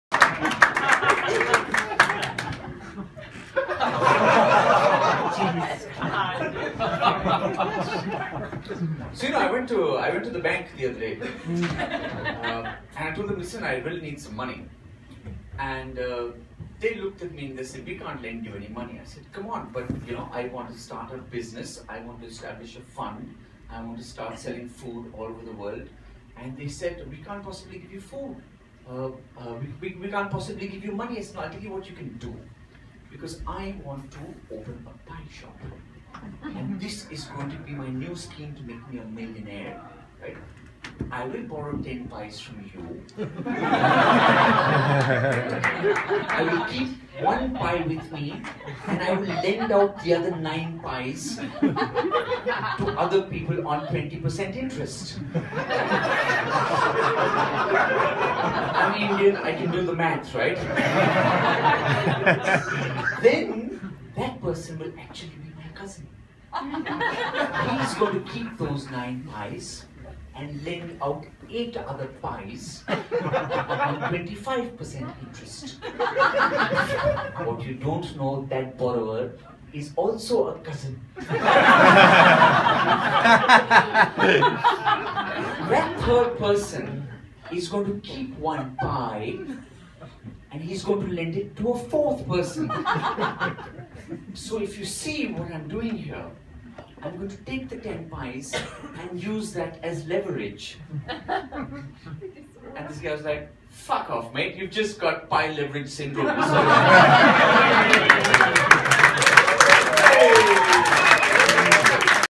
Indian comedian Papa CJ's debut on SETLIST at the Melbourne International Comedy Festival
My topic was 'Pie Leverage Syndrome Disorder'!

It’s raw, it’s panic-inducing and for those about to take the stage, Set List’s “Stand-Up Without A Net” can feel like the comedic equivalent of stepping into the ring against Muhammad Ali. It’s been described by several comedians as the most difficult gig they’ve ever done. They also can’t stop talking about it.

Co-produced by Los Angeles comedians Troy Conrad and Paul Provenza, the format forces comedians to take the stage without any pre-written material. Their set will instead stem from three to four irreverent subjects displayed alternately on a screen behind them. For example: “Introverted Dictator,” or “A Misogynistic Serenade,” or even “Bestiality Glory Hole Roulette.” The comic is exposed to the off-beat topics at the same time as the audience, and they’re not allowed to use any material they’ve performed before. They get booed if they do. Conrad and Provenza describe “Stand-Up Without a Net” as “asking a magician to perform real magic.”